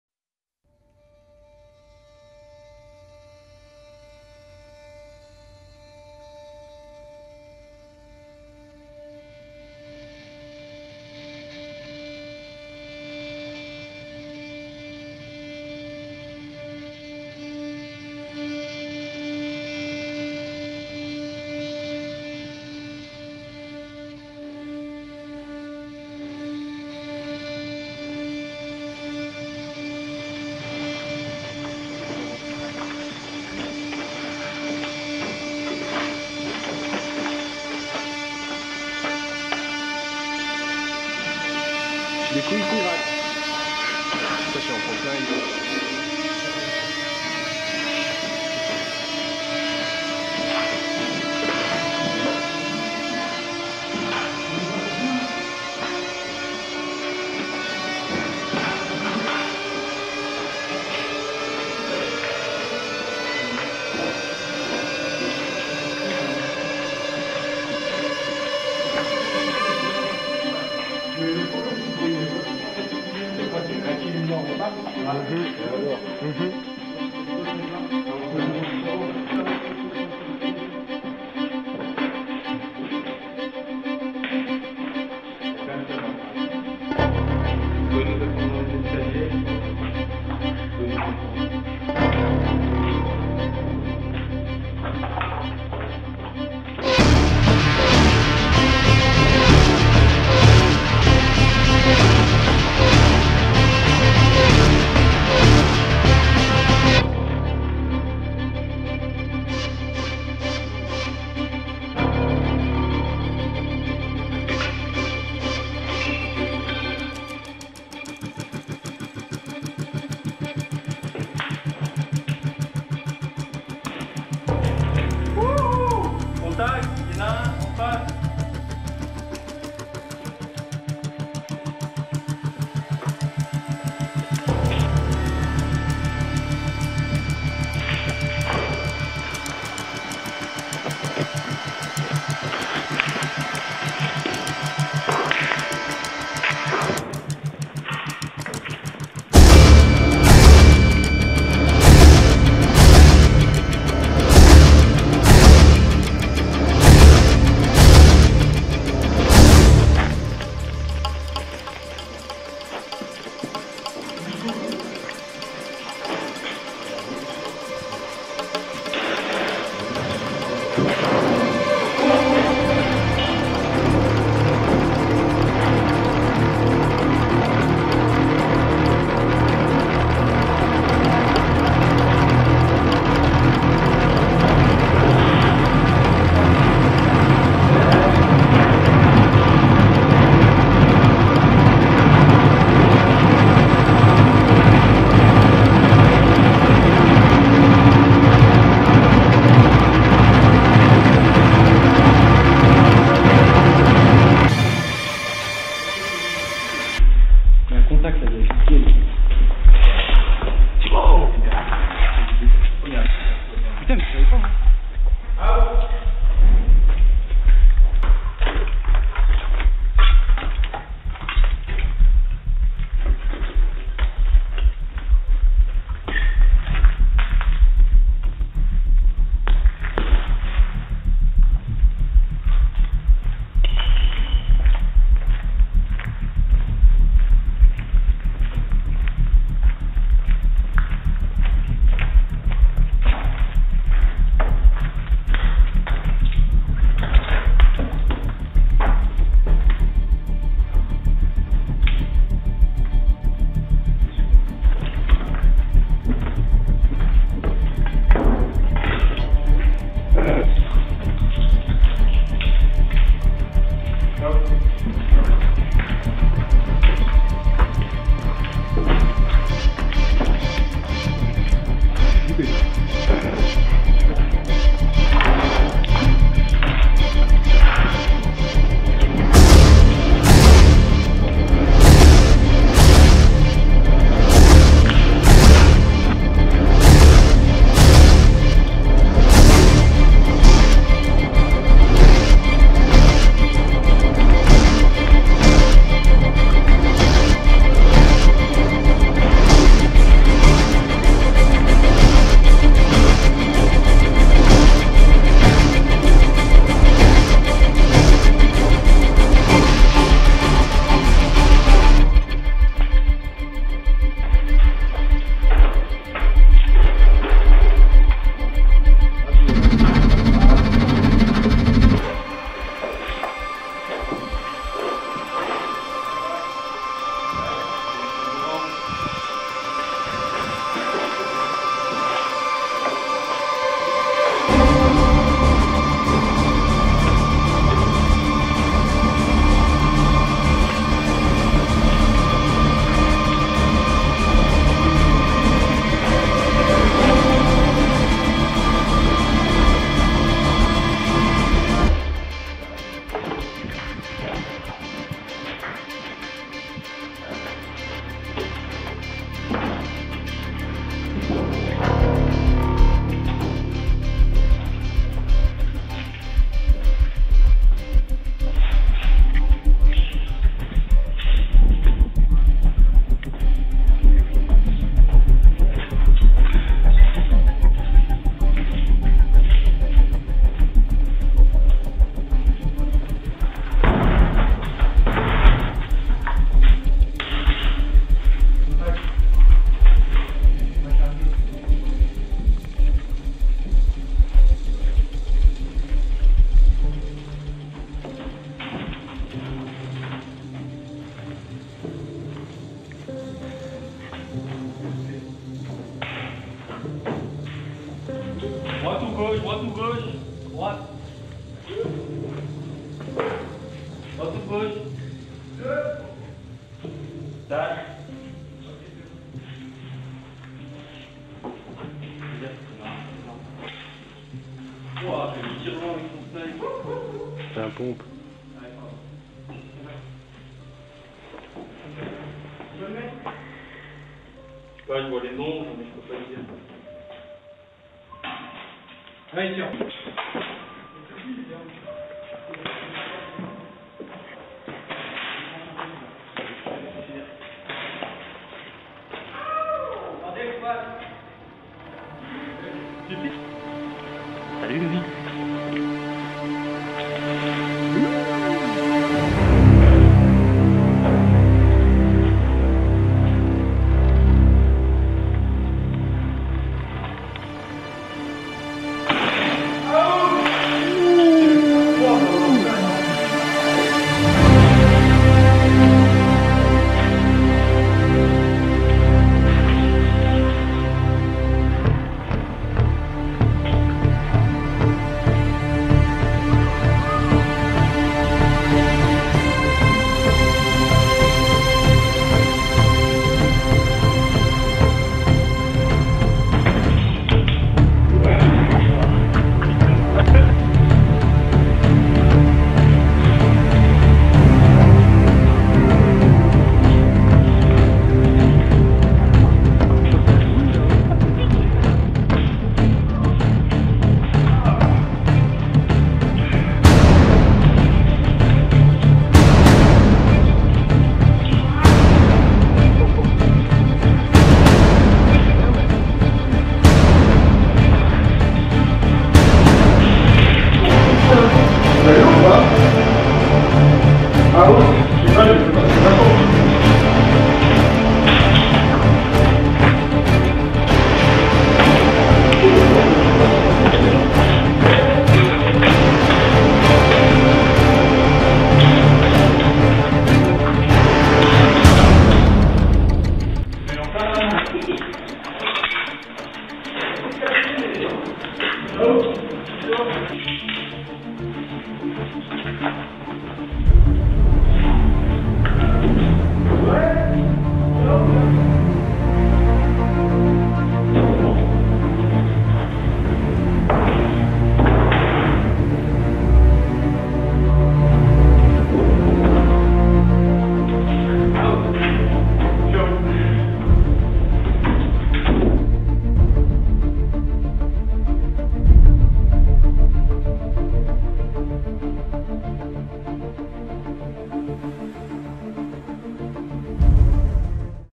GKAT St-Hilaire CQB-Pistol Only
Partie du 12/08/12 a St-Hilaire du touvet (50000 m2 de cqb)
Facebook: # Generation Kill Airsoft Team